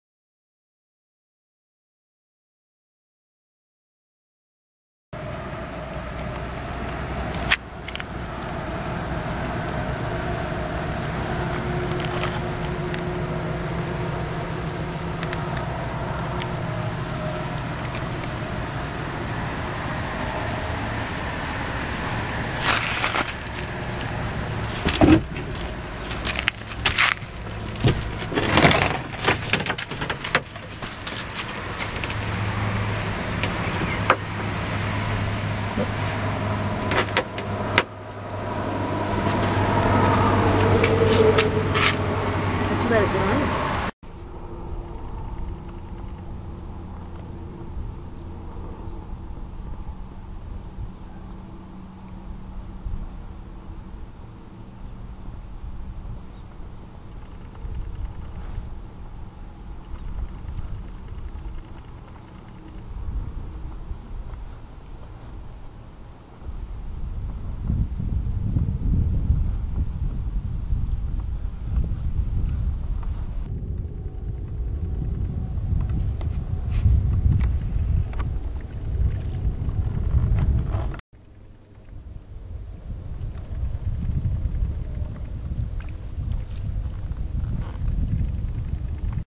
Leeching a Ride
After kayaking on Messalonski Lake in Belgrade, Maine, we discovered these "hitchhikers." (Clips of Pied-billed Grebe with young, and Common Loon, provided to erase what for some viewers may be nightmarish images of hungry leeches!)